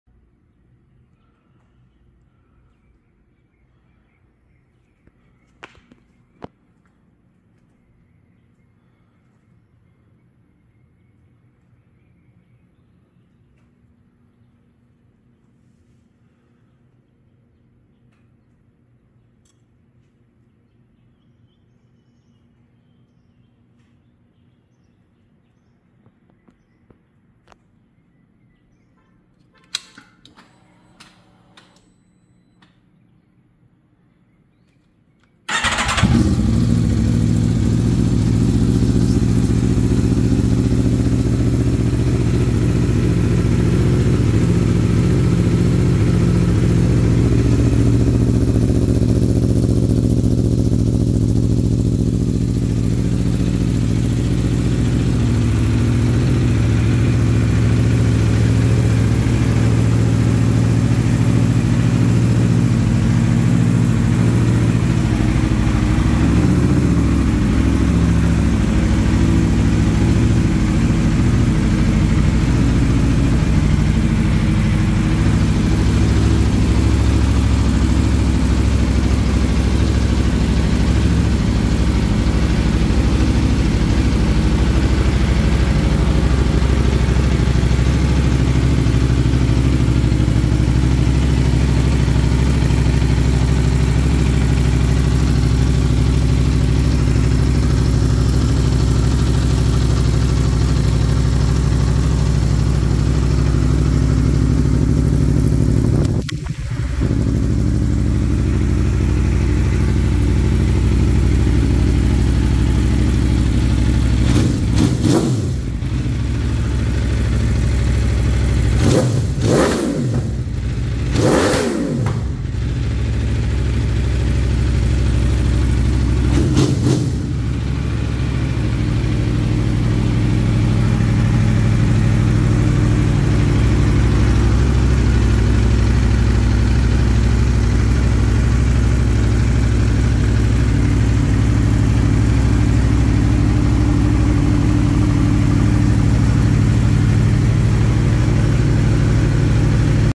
Aprilia RSV4. What kind of bike do you ride?
Please like and sub please.

Hope you enjoy my bike walkaround.

It does have the basic mods.

Aprilia ECU race map.
Air Filter
Akro CF exhaust.
Stealth mirrors.
Frame sliders.
Rear sets.
Tail delete.

I don't ride much which is a shame.  Bike has around 300 miles on it and I've had it for almost a year now.